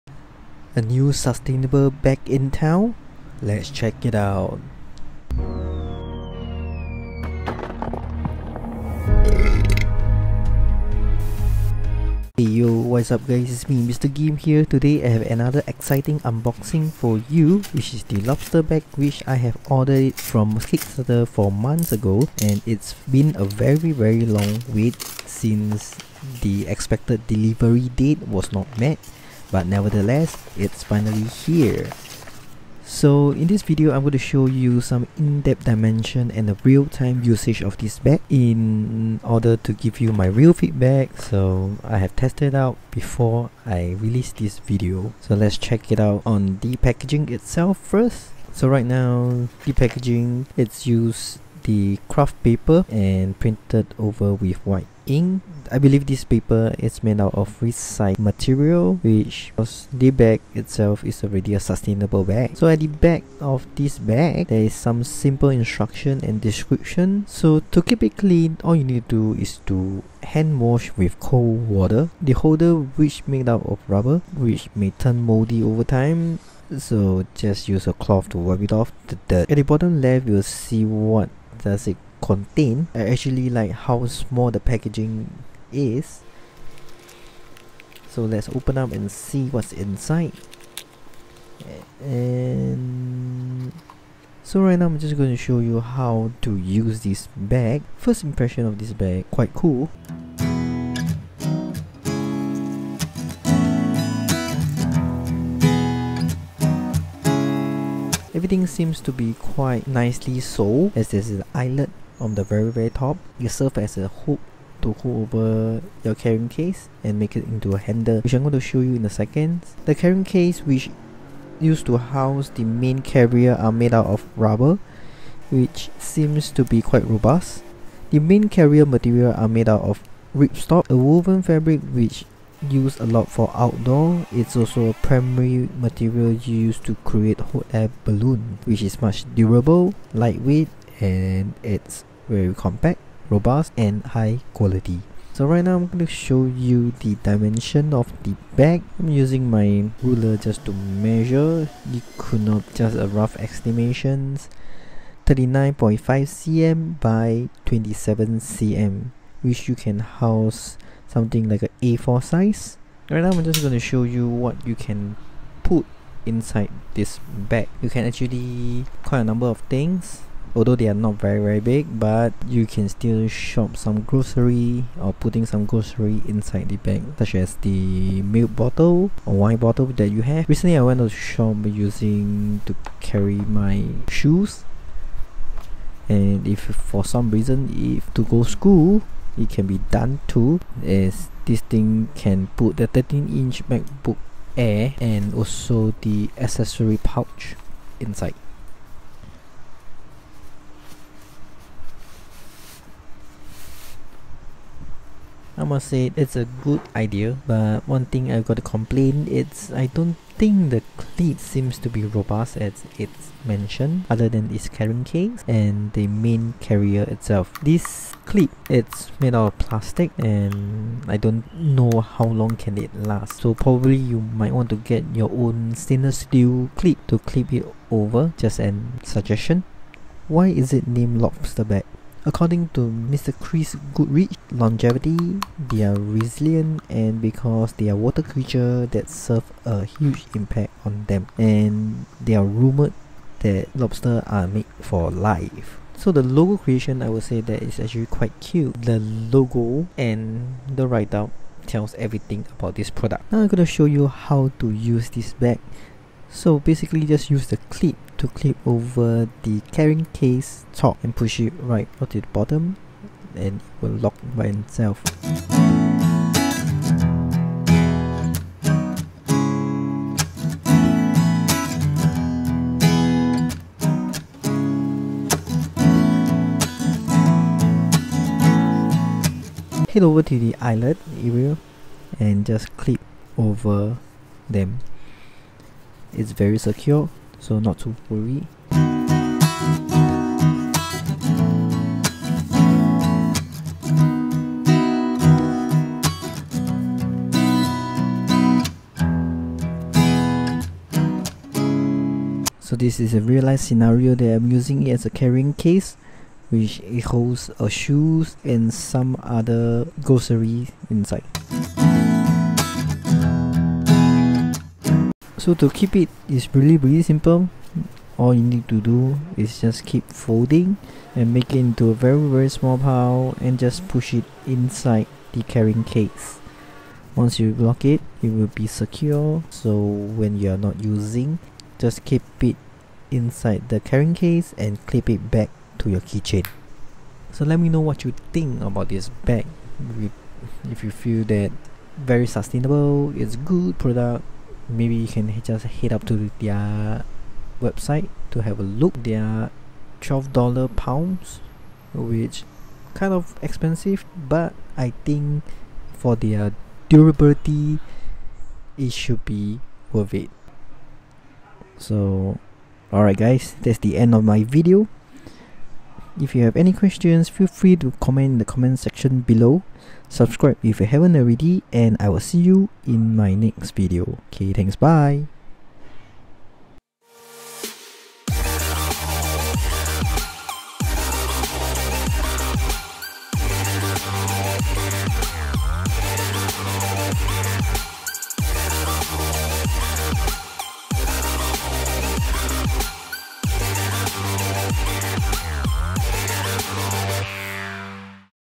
LobsterBag | The World's Most Compact Key-Fob Bag Gadget
In today's video i'm going to share with you guys the LobsterBag, probably one of the most
highly compact and user friendly stylish Bag.
Practically small, amazing functionality without taking up too much space. Kind of difference from the rest of the reusable bag. and is easily repacked.